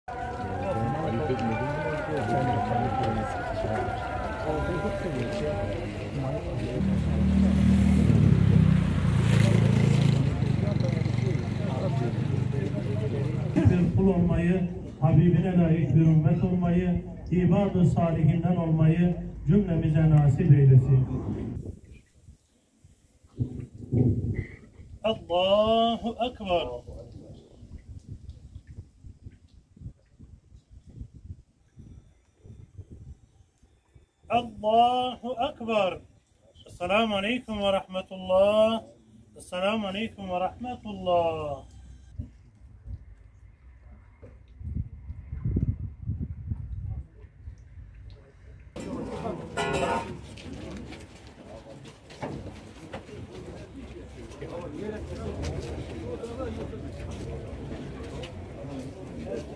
Konya'da saman tozu kavgasında hayatını kaybeden karı koca son yolculuğuna uğurlandı
📢 Güncel haberlerden anında haberdar olmak için;